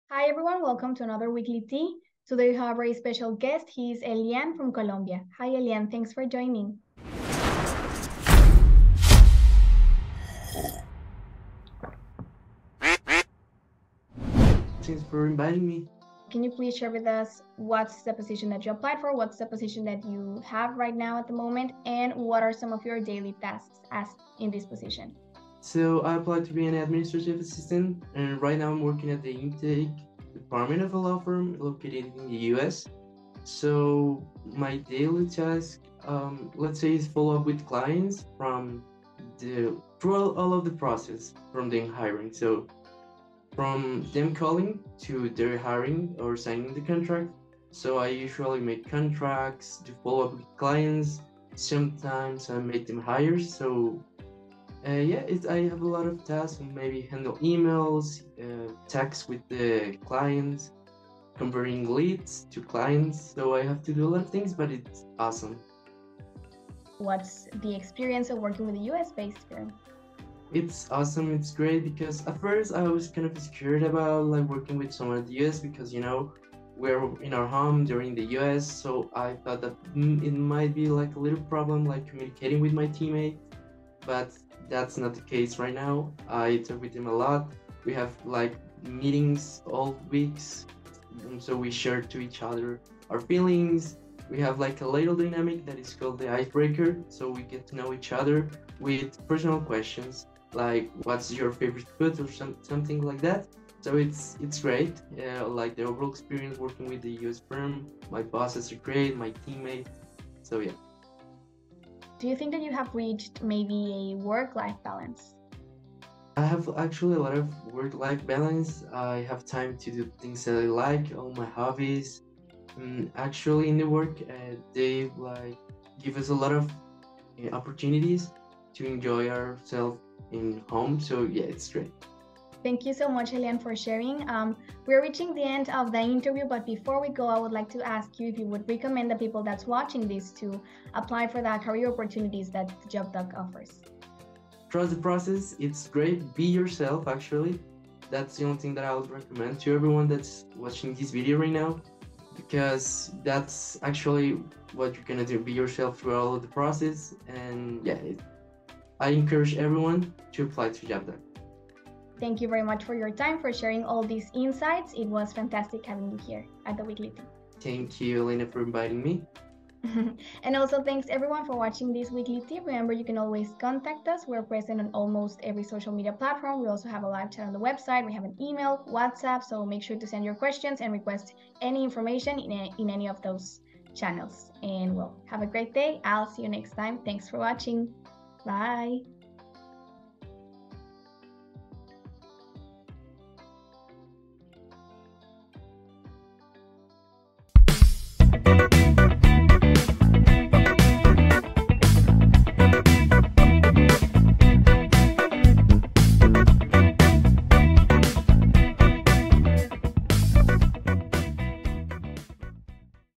Weekly Tea with Elian from Colombia
⭐️ You were an incredible guest, Elian.

📌Elian is from Colombia

⭐️Elian applied for the Administrative/Executive Assistant role. Right now, he is working in an Intake role. His daily tasks are: following up with clients, making contracts, handling emails, and hiring.

❤️Elian thinks his experience has been awesome. He communicates well with his teammates. He also feels he has reached a work-life balance.